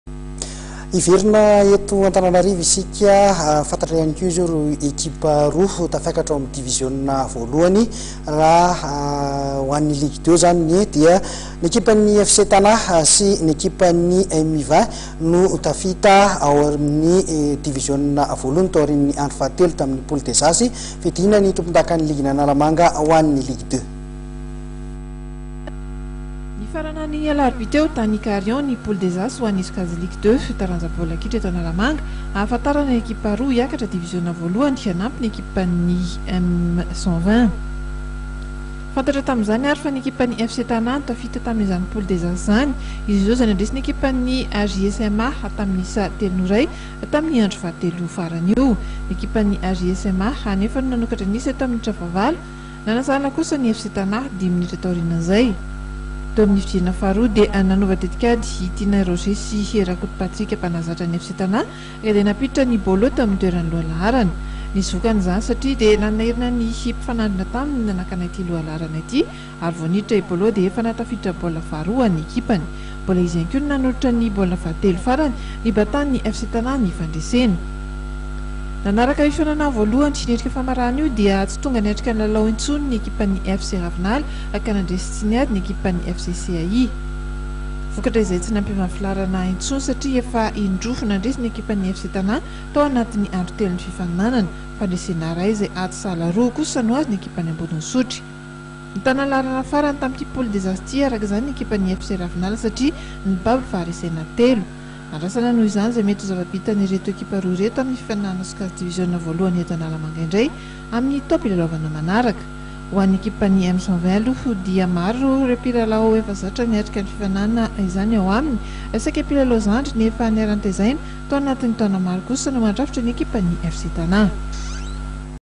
4 FOOT LEAGUE2
Ny ekipan'ny FC Tana no hanampy ny ekipan'ny M 120 hiakatra ao amin'ny diviziona voalohany baolina kitra